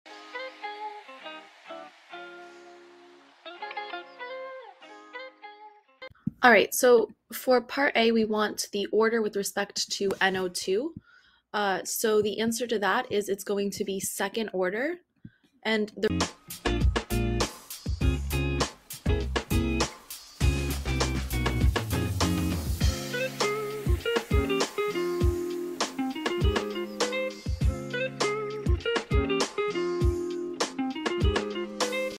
The following data were obtained for the gas phase decomposition of nitrogen dioxide at 300Â°C: NO2…
The following data were obtained for the gas phase decomposition of nitrogen dioxide at 300Â°C: NO2(g) â†’ NO(g) + 1/2O2(g) Time (s) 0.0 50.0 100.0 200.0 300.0 [NO2] (M) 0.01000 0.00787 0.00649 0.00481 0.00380 Find the reaction order with respect to NO2(g). What is the value of the rate constant? What is the half-life time when the initial concentration is 1 mol/L?